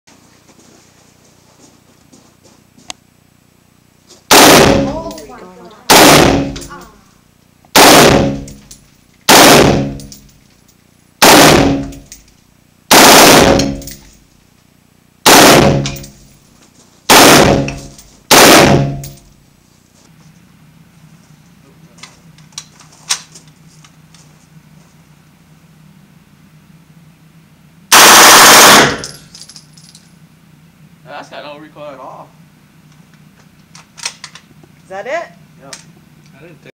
Full Auto M16 and HKMP5
Shooting custom auto guns. If you dont own one or haven't shot one I suggest you do it.